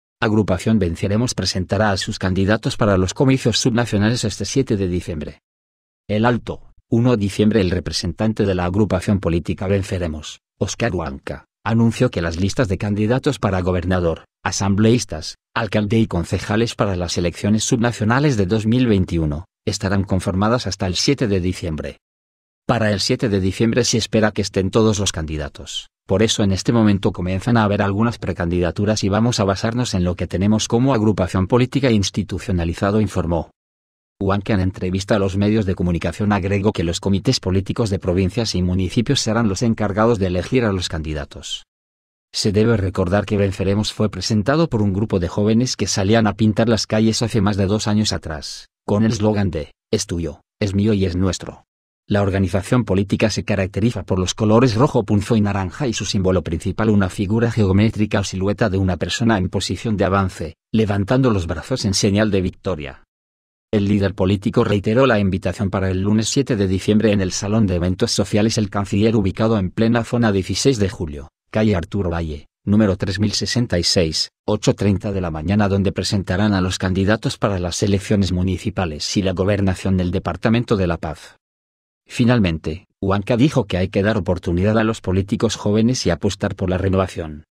Agrupación Venceremos presentará a sus candidatos para los comicios subnacionales este 7-12
El representante de la Agrupación política Venceremos, Oscar Huanca, anunció que las listas de candidatos para gobernador, asambleístas, alcalde y concejales para las elecciones subnacionales de 2021, estarán conformadas hasta el siete de diciembre.

"Para el siete de diciembre se espera que estén todos los candidatos, por eso en este momento comienzan a haber algunas precandidaturas y vamos a basarnos en lo que tenemos como Agrupación política e institucionalizado" informó.

Huanca en entrevista a los medios de comunicación agregó que los comités políticos de provincias y municipios serán los encargados de elegir a los candidatos.

Se debe recordar que Venceremos fue presentado por un grupo de jóvenes que salían a pintar las calles hace más de dos años atrás, con el slogan de: “Es tuyo, es mío y es nuestro”. La organización política se caracteriza por los colores “rojo punzo y naranja” y su símbolo principal una “figura geométrica o silueta de una persona en posición de avance, levantando los brazos en señal de victoria”.

El líder político reiteró la invitación para el lunes siete de diciembre en el salón de eventos sociales "El Canciller" ubicado en plena zona 16 de Julio, calle Arturo Valle, número 3066, 08:30 de la mañana donde presentarán a los candidatos para las Elecciones Municipales y la Gobernación del Departamento de La Paz.

Finalmente, Huanca dijo que hay que dar oportunidad a los políticos jóvenes y apostar por la renovación.